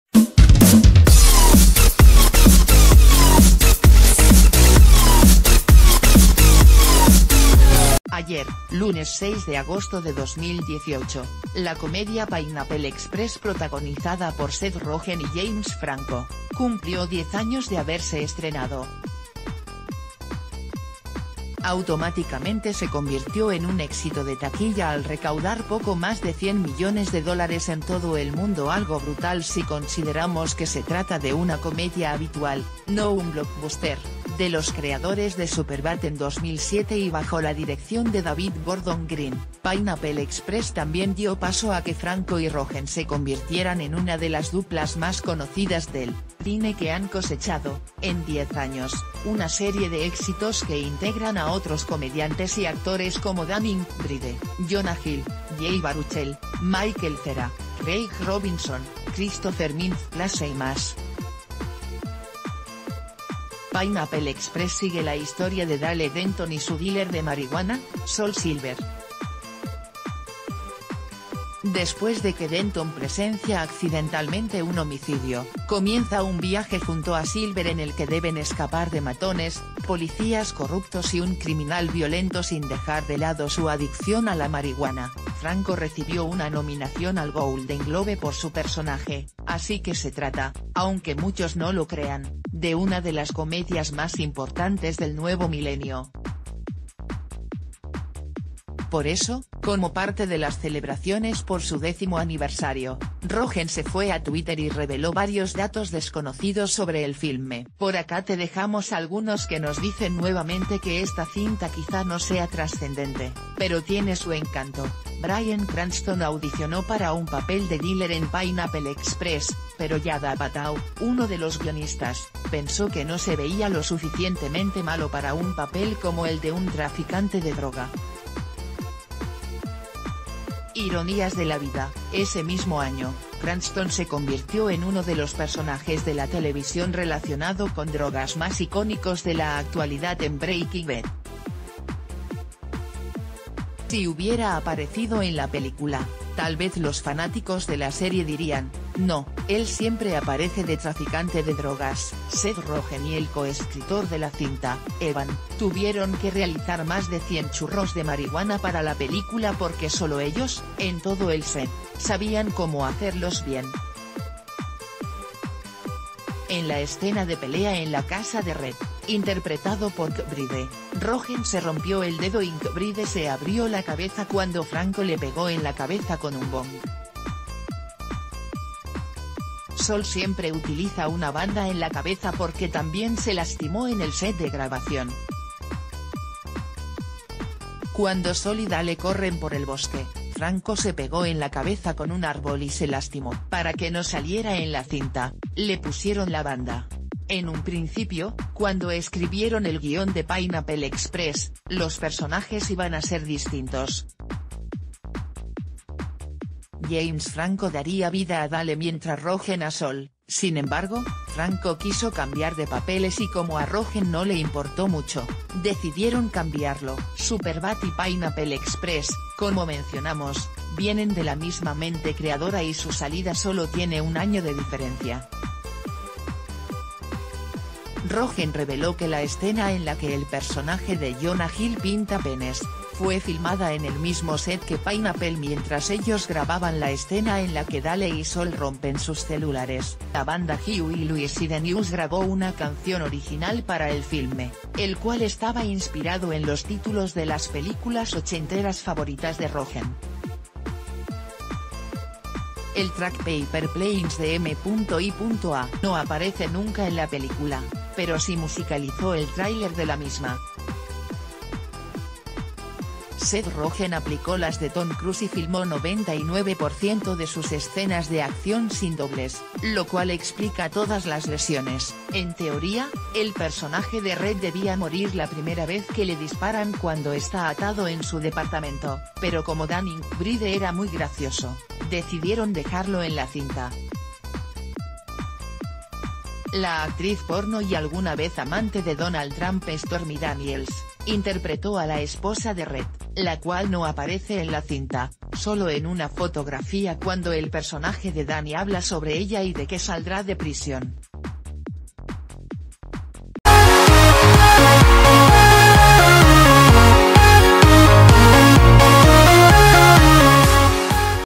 10 cosas que no sabías de Pineapple Express por su décimo aniversario
10 cosas que no sabías de Pineapple Express por su décimo aniversario
Ayer, lunes 6 de agosto de 2018, la comedia Pineapple Express protagonizada por Seth Rogen y James F...